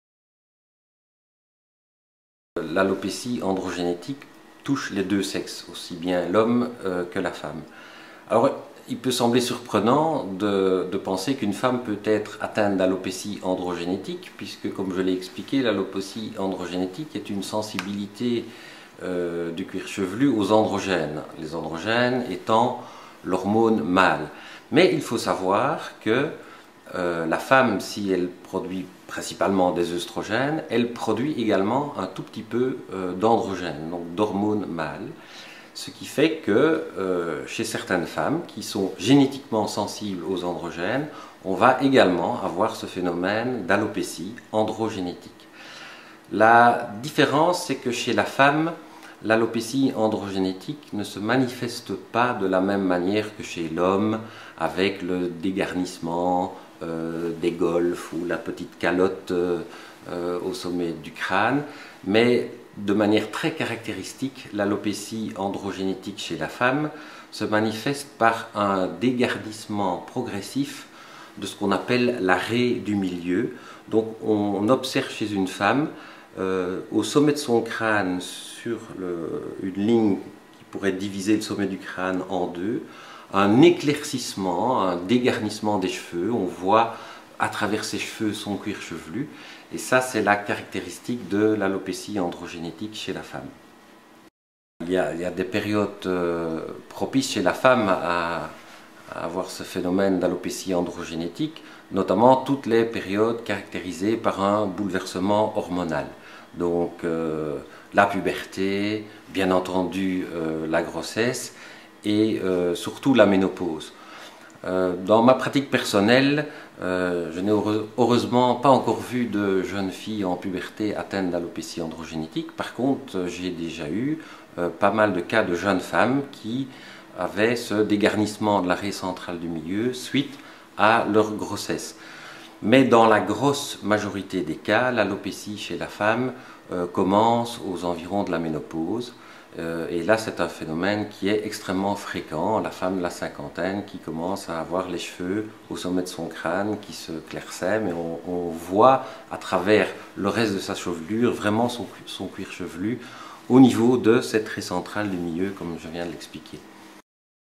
Solutions anti chute de cheveux par Thierry Schmitz - alopécie femme
Thierry Schmitz ,naturopathe diplomé de la faculté de médecine de l'UCL Belgique, vous parle de l'alopecie chez la femme